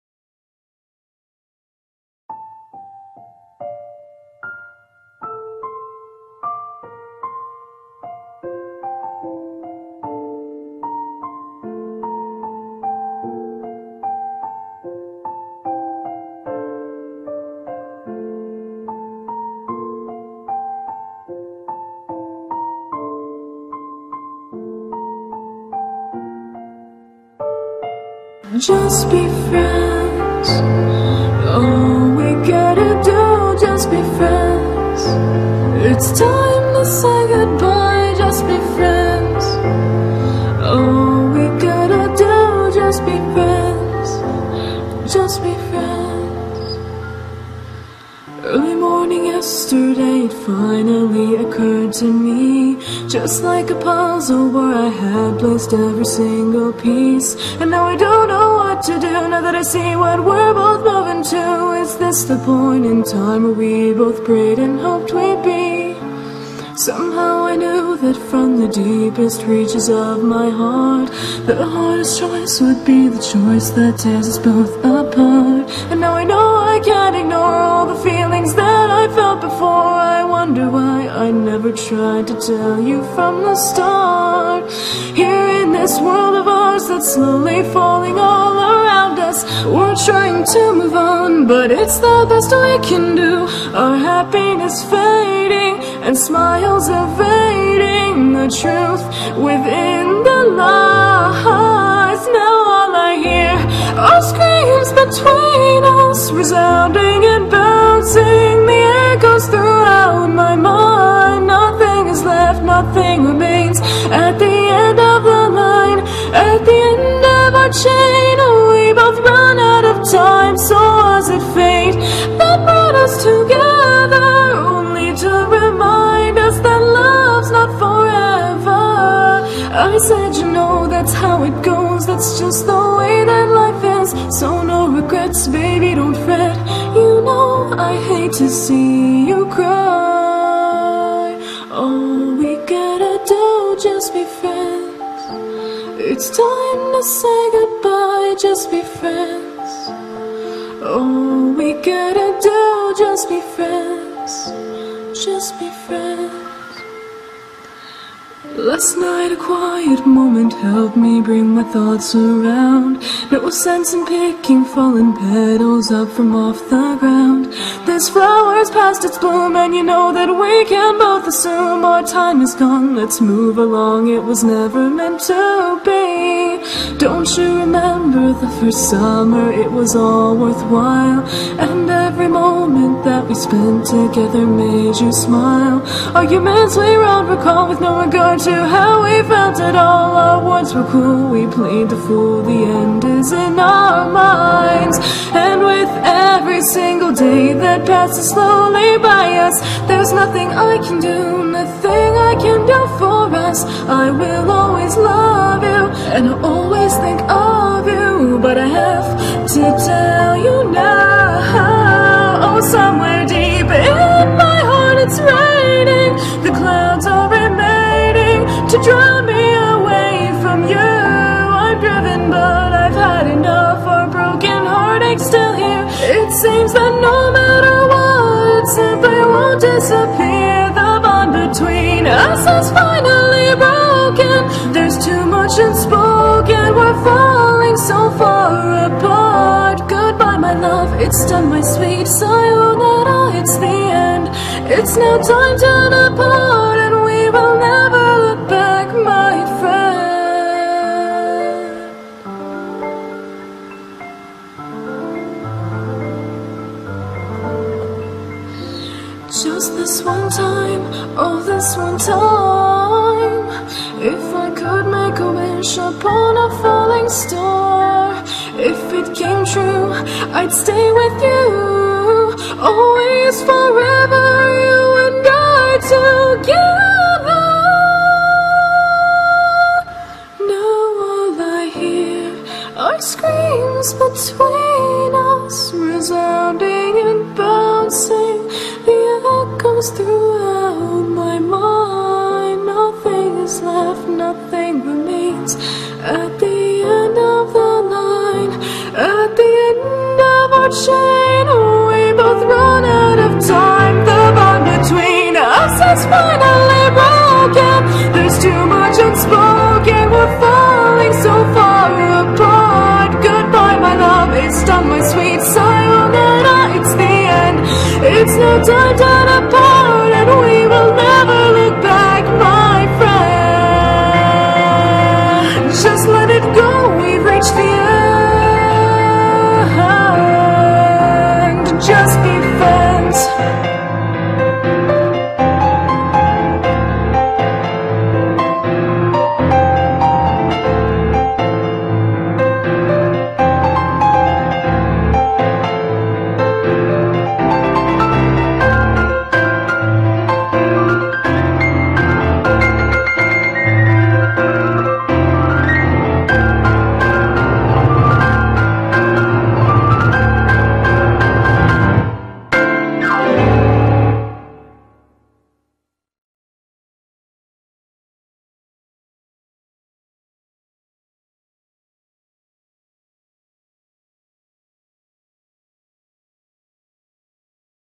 [homestuck - john x vriska] just 8e friends.
There is nothing I have to tell you other than "whoops, accidentally sadstucked". Blame it on an unavoidable case of the 'feels'.